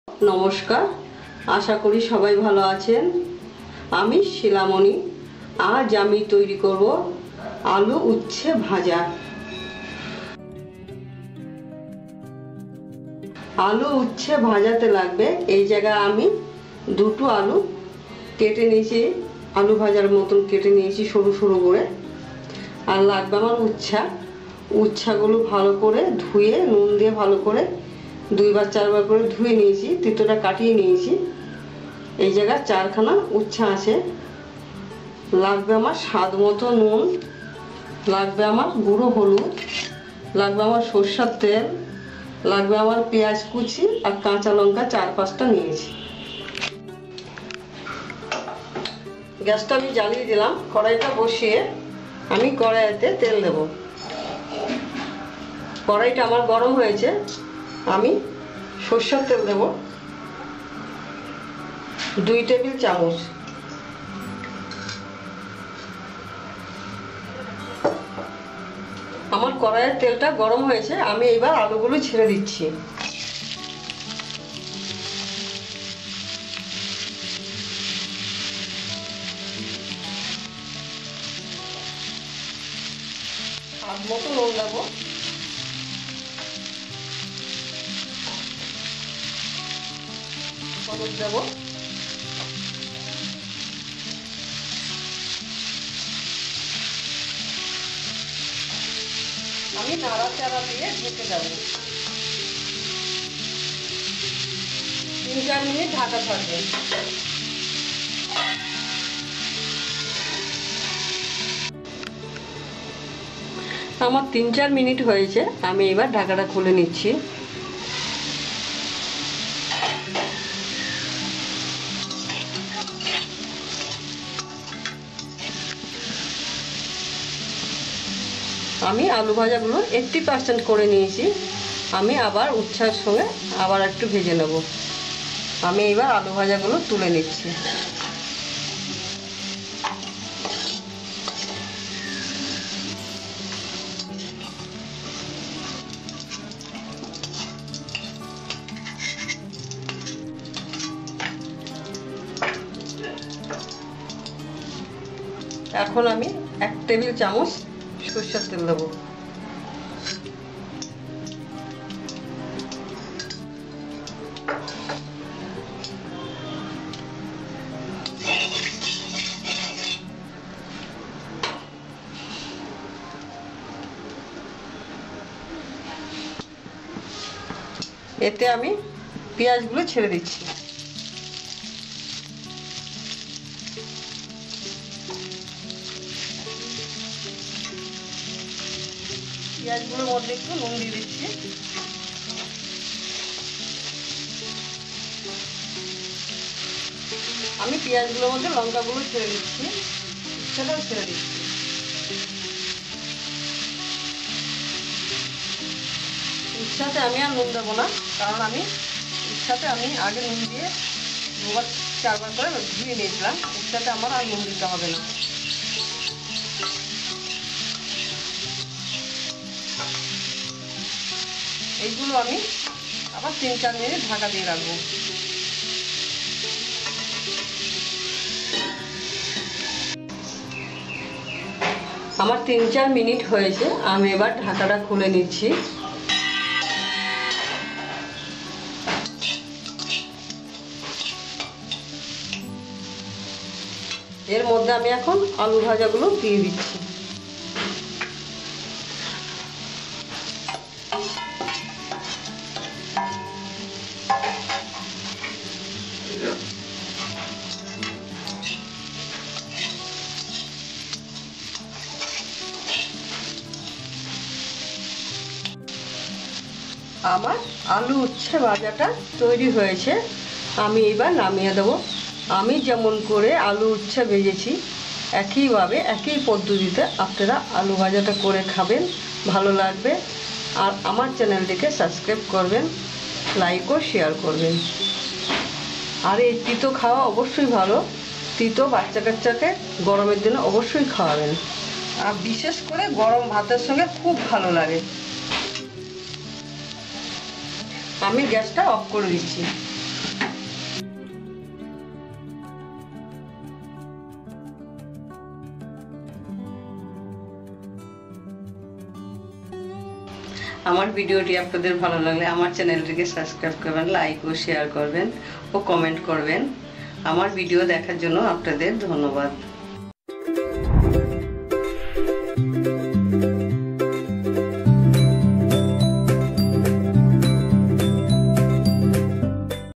খুব সহজ ও মজাদার উচ্ছে আলু ভাজা | Ucche Aloo Bhaja Recipe | Bengali Style Recipe
হ্যালো বন্ধুরা ! আমার চ্যানেলে আপনাকে স্বাগতম! এই পর্বের মধ্যে আমি "উচ্ছে আলু ভাজা" তৈরি করব।
লাইক কমেন্ট ও শেয়ার করে সাথেই থাকুন। দেখতে থাকুন আমাদের প্রোগ্রাম।